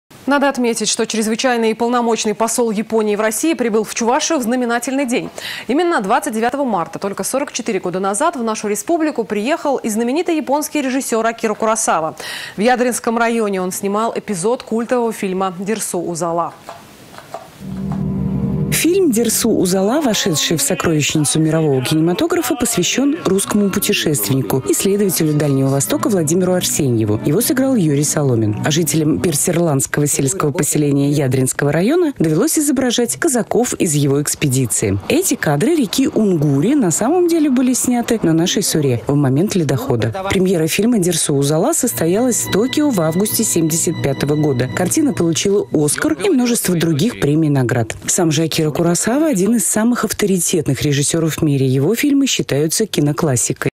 29 марта 44 года назад в нашу республику приехал и знаменитый японский режиссер Акира Куросава.
Надо отметить, что Чрезвычайный и Полномочный Посол Японии в России прибыл в Чувашию в знаменательный день. Именно 29 марта, только 44 года назад в нашу республику приехал и знаменитый японский режиссер Акира Куросава. В Ядринском районе он снимал эпизод культового фильма Дерсу Узала.